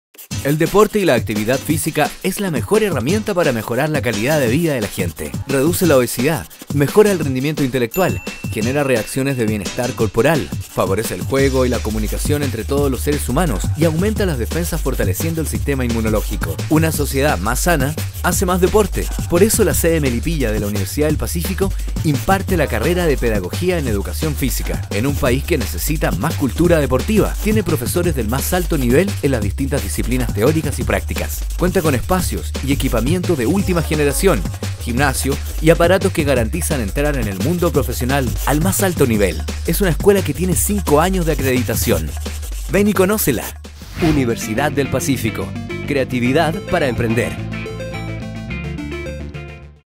Pedagogía en Educación Física - Universidad del Pacífico
Universidad del Pacífico - Creatividad para Emprender
FACULTAD DE CIENCIAS HUMANAS Y EDUCACIÓN
Pedagogía en Educación Física

Educación, deporte, prevención y calidad de vida

Formando profesionales capaces de desempeñarse en las áreas de educación, deporte, prevención en salud y calidad de vida. Se ha requerido asumir y flexibilizar la orientación de las competencias y habilidades que le posibiliten desenvolverse exitosamente en el profesional actual y futuro. La pertinencia de esto radica en los avances que la Educación Física ha tenido en el plano de la prevención a enfermedades crecientes en la población nacional, tales como; obesidad, hipertensión y patologías cardíacas.

Sede Melipilla
Avenida José Massoud 533, Melipilla, Chile